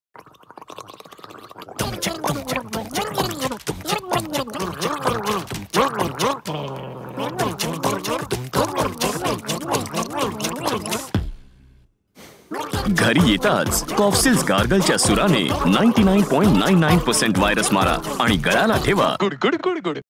Cofsils Gargle for Good Health | CIPLA | 20 Seconds | MARATHI
No germs = No diseases
Gargle your way to good health by making it a habit to use Cofsils Gargle before and after leaving home. Cofsils Gargle kills 99.99% germs*
Just pour some gargle and get gudgud-ing! Stay safe, stay healthy.

*Infect Dis Ther 2018; 7:249-259